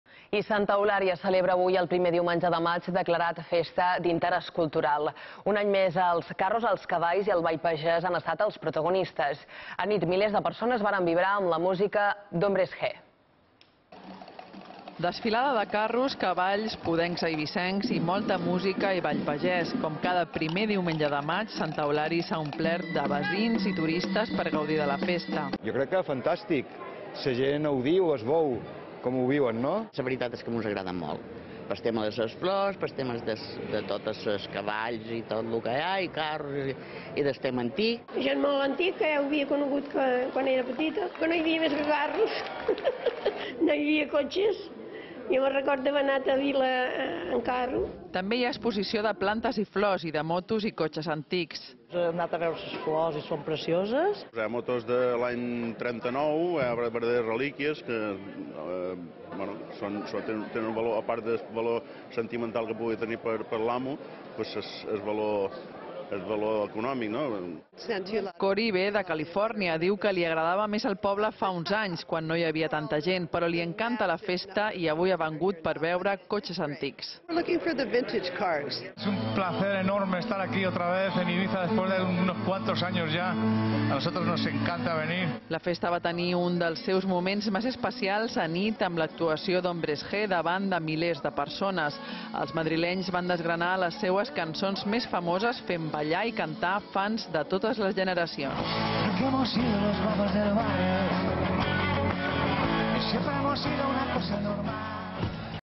Carros, ball pagès i Hombres G per celebrar el Primer Diumenge de Maig de Santa Eulària
Santa Eulària celebra avui el Primer Diumenge de Maig, declarat Festa d'Interès Cultural. Un any més els carros, els cavalls i el ball pagès han estat els protagonistes. Anit, milers de persones varen vibrar amb la música d'Hombres G.